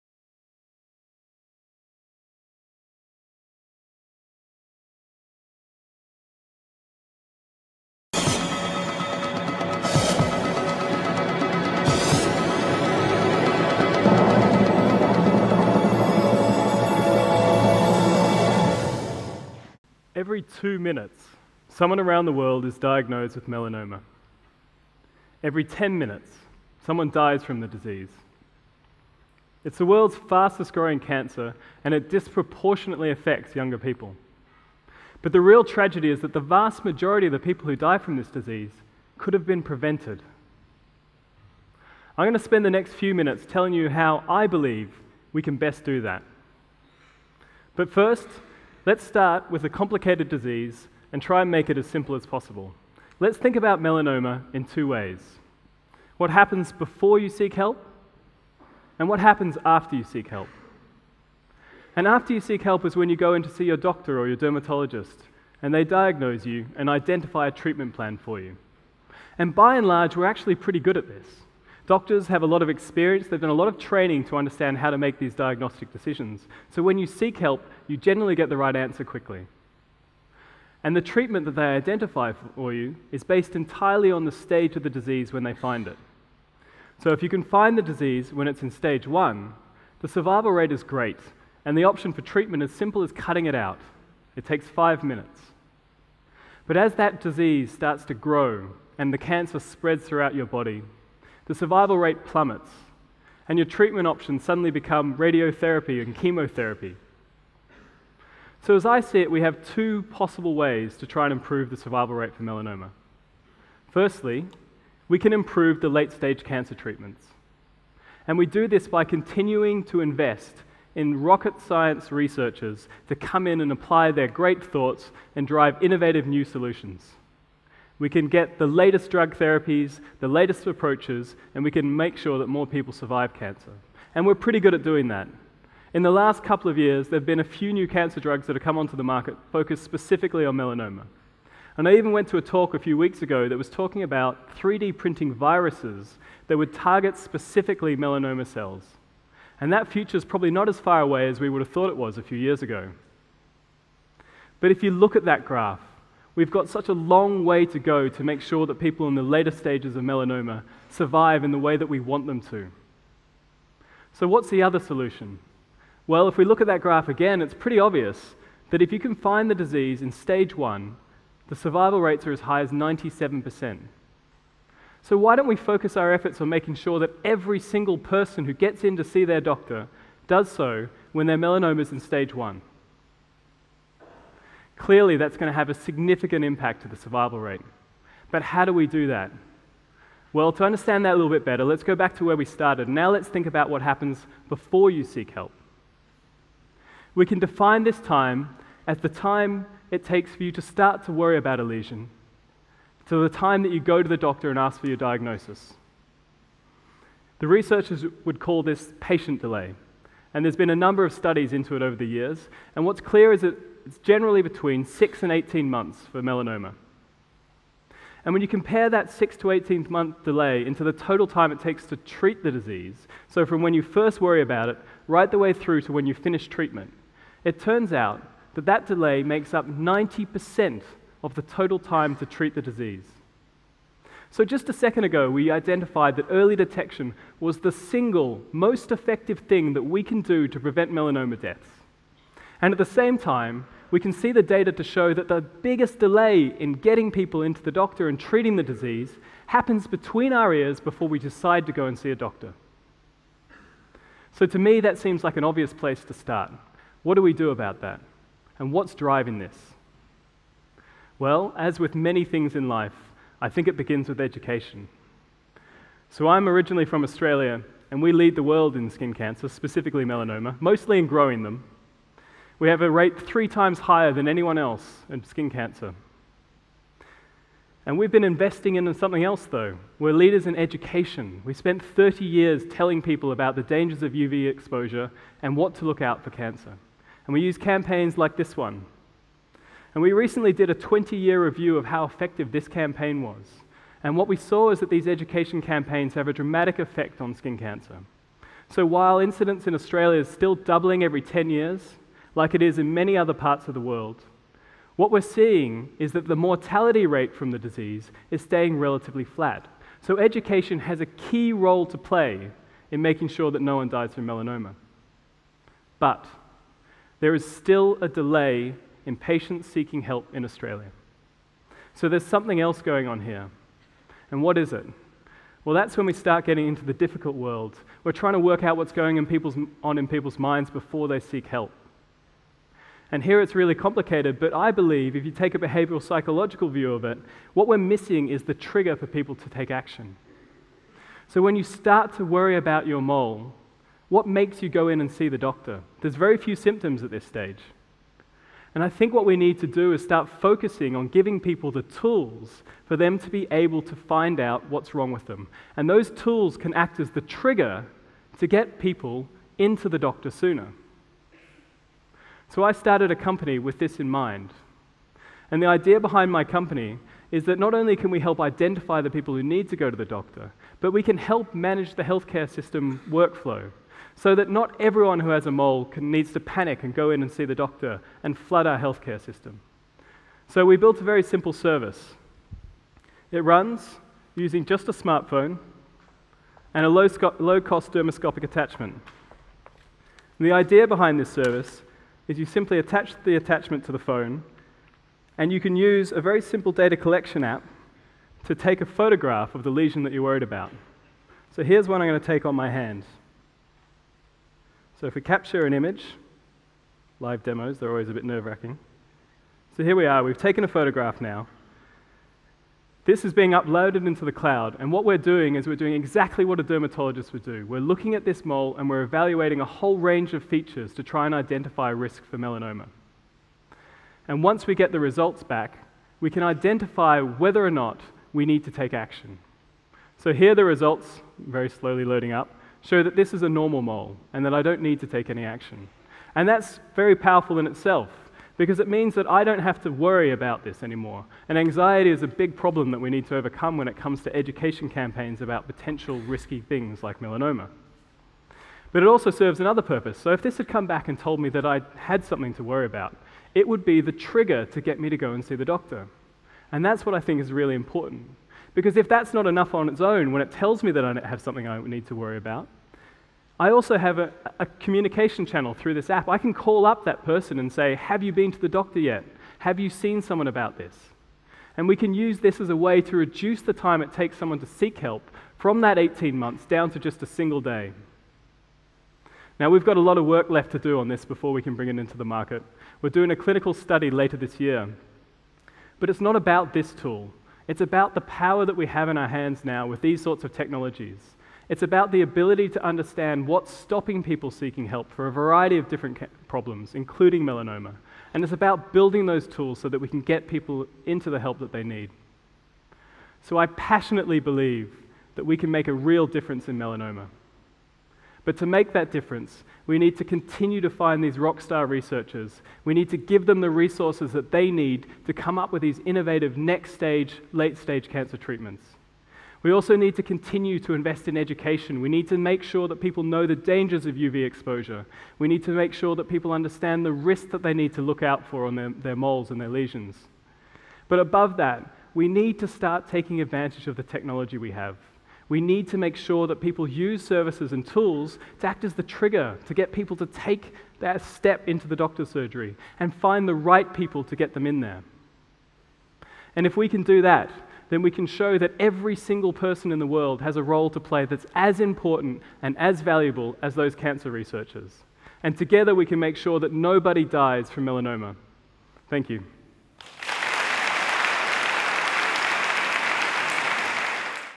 Making Melanoma Deaths a Thing of the Past | Neil Daly | TEDxWarwick
Neil Daly is an avid follower of innovative health solutions and founder of Skin Analytics, a service which uses an attachment to turn a smartphone into a melanoma diagnostic service. With melanoma the fastest growing cancer worldwide, Skin Analytics provides a low cost solution to identify the disease earlier when treatment is significantly more effective and significantly cheaper - it’s a powerful tool that could - literally - make the difference between life and death!

Previously, Neil worked with mobile operators in emerging markets to build mobile money services, providing financial service access to those at the base of the economic pyramid. Neil was also involved in setting up the the worlds first mobile remittance hub.

Neil studied Physics at the University of Western Australia before moving to the UK and completing and Executive MBA at London Business School.